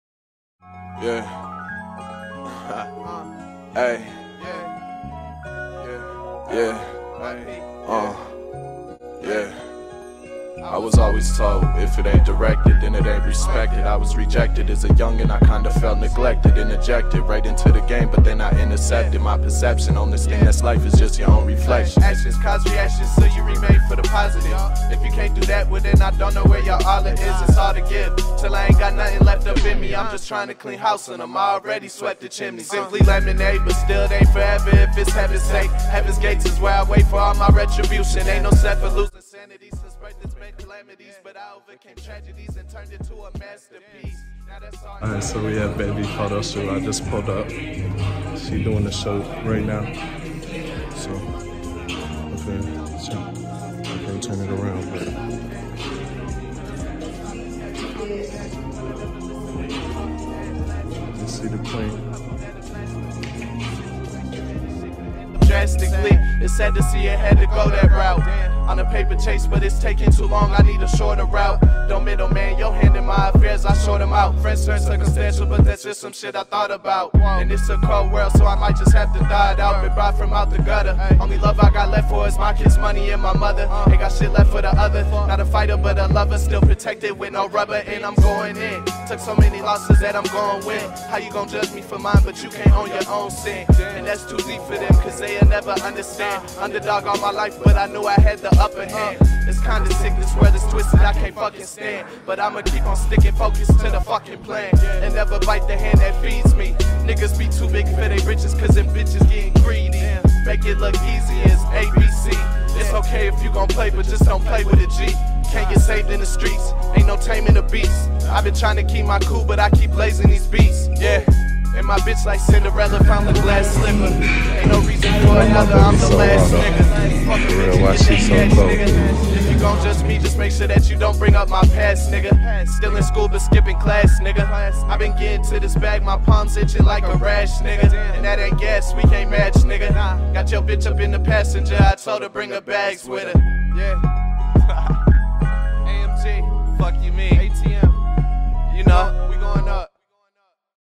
Buns & Guns Rach: B-ROLL VIDEO
Issa PHOTOSHOOT

Behind the scenes shooting with;
MN EXPRESS SOCIAL @mnexpresssocial
The lovely owners Lauren & Desmond Brooks
@photosby_lolob x @thecinematicdude

Make up & styling by myself & Lauren

BTS VID & MUSIC  by @atm_yp

Video edit: myself @bunsnguns_rach
Can’t wait to see how these photos turn out 💖

Book me for modeling, styling or posing help — virtual training & more